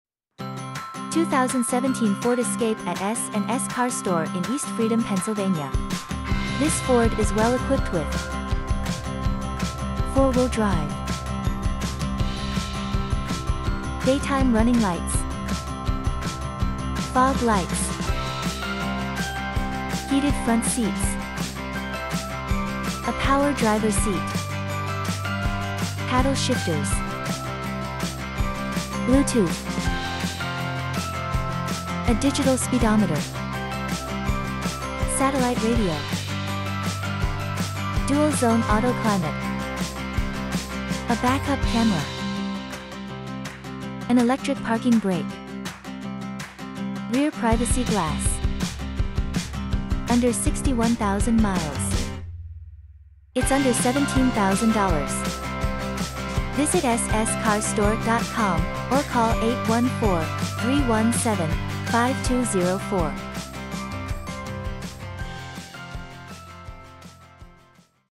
2017 FORD ESCAPE at S+S Car Store in East Freedom, PA - B00011
2017 FORD ESCAPE  ***  4WD – Backup Camera – Dual Zone Auto Climate – Heated Front Seats – Power Driver Seat – Bluetooth – Satellite Radio – Digital Speedometer – Rear Privacy Glass – Paddle Shifters – Electric Parking Brake – Daytime Running Lights – Fog Lights - Under $17K – Under 61K Miles